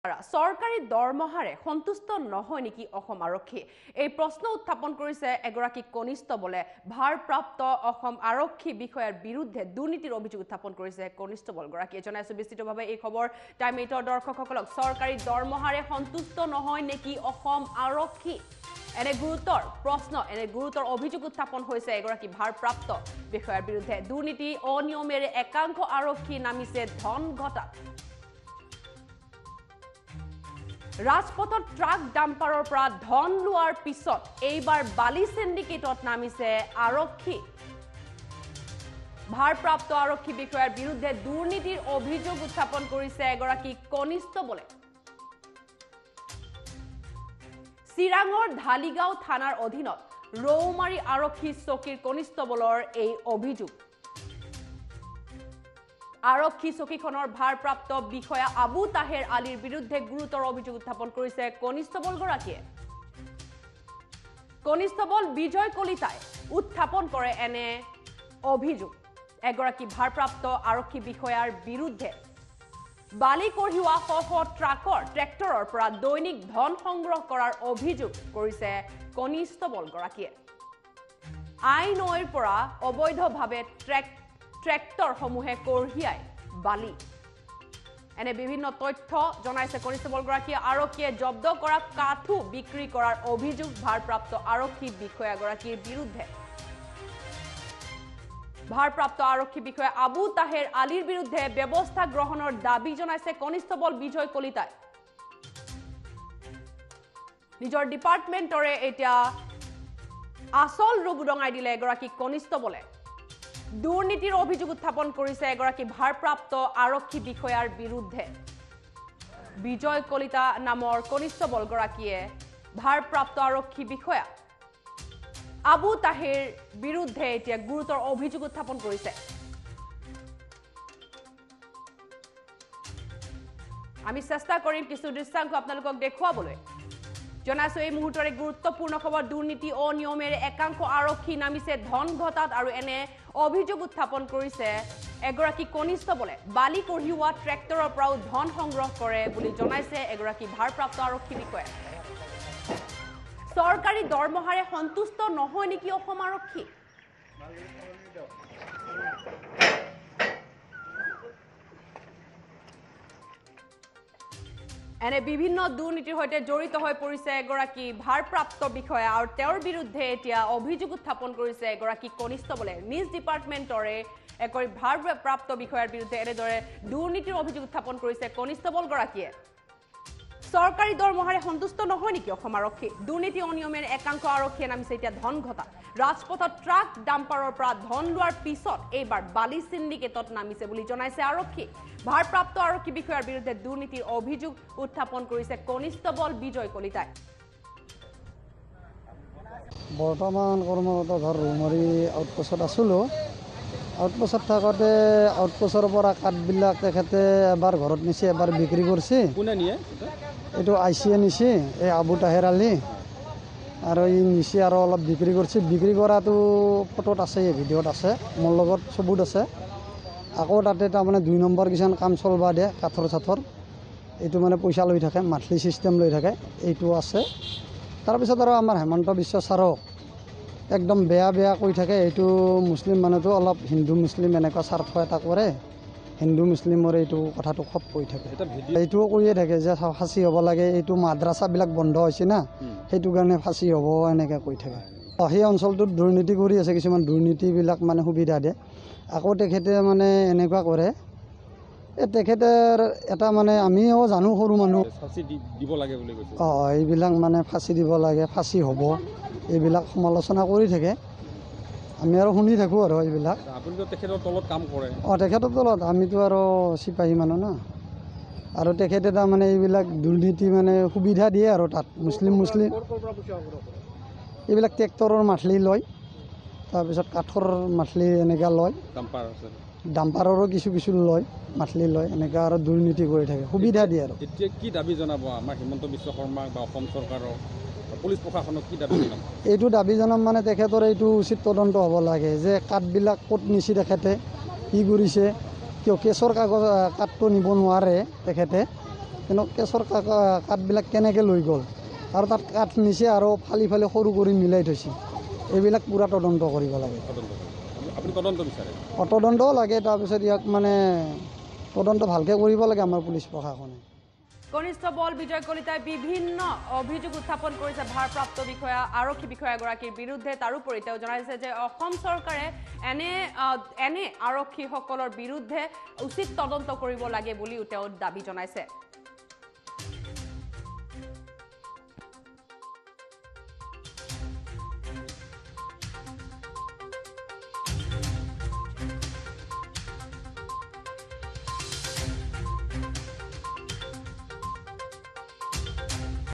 ৰাজপথত ট্ৰাক-ডাম্পাৰৰ পৰা ধন লোৱাৰ পিছত এইবাৰ বালি চিণ্ডিকেটত আৰক্ষী I
চৰকাৰী দৰমহাৰে সন্তুষ্ট নহয় নেকি অসম আৰক্ষী? ৰাজপথত ট্ৰাক-ডাম্পাৰৰ পৰা ধন লোৱাৰ পিছত এইবাৰ বালি চিণ্ডিকেটত আৰক্ষী I ভাৰপ্ৰাপ্ত আৰক্ষী বিষয়াৰ বিৰুদ্ধে দুৰ্নীতিৰ অভিযোগ কনিষ্টবলৰ I